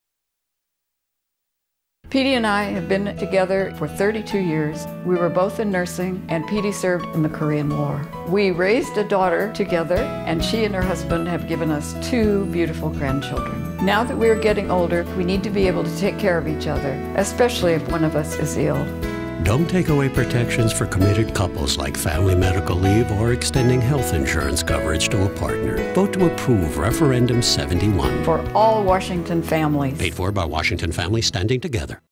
TV Ad: Together
The ad features Jane & Pete-e a committed couple  whove shared their lives for 32 years.